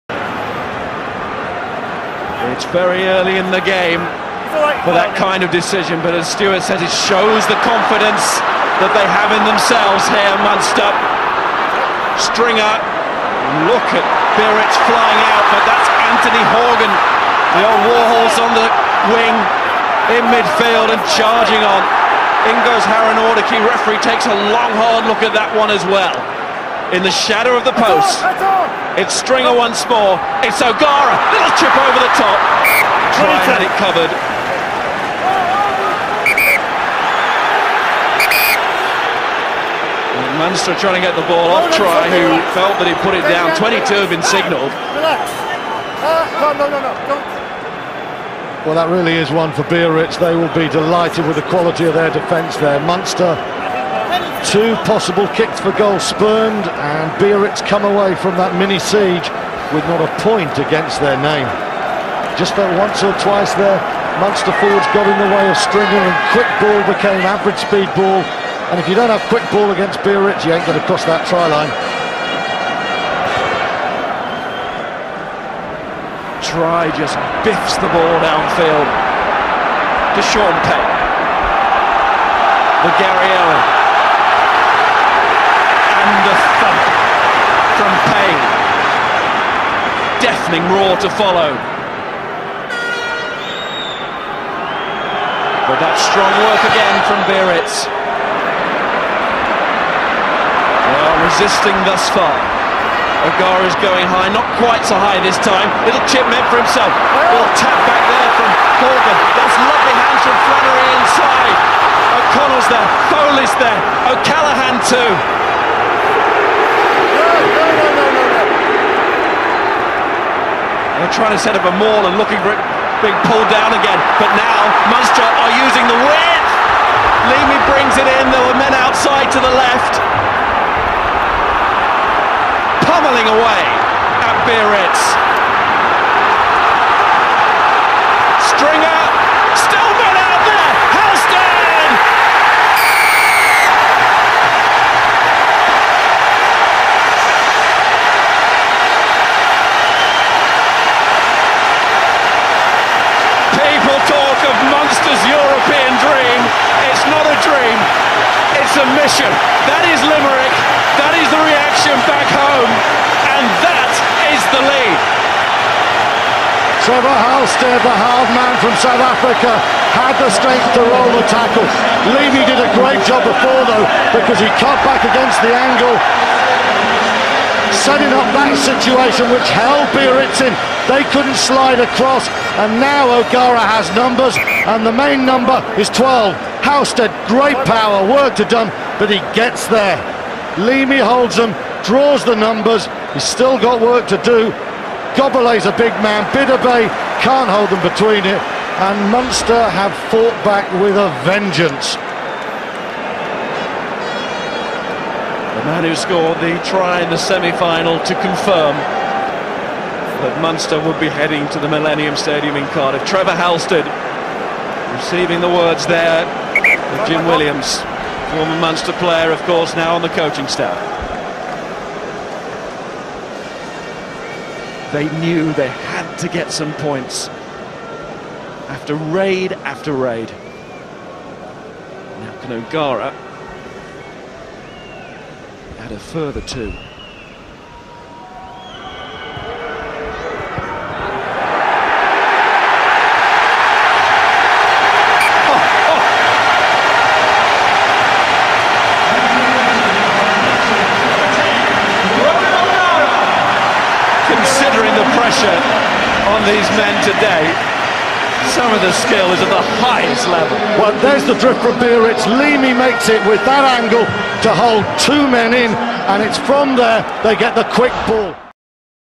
Trevor Halstead scores Munster Rugby's first try against Biarritz in the 2006 Heineken Cup Final
Three HeinekenChampionsCup finals later, Munster finally took home their first title in 2006.

Trailing Biarritz after the first 15 minutes, Munster needed a try to keep the title in their sights. Trevor Halstead, the South African centre, crashed over the line giving the red army their first score of the match to set up a dramatic title decider.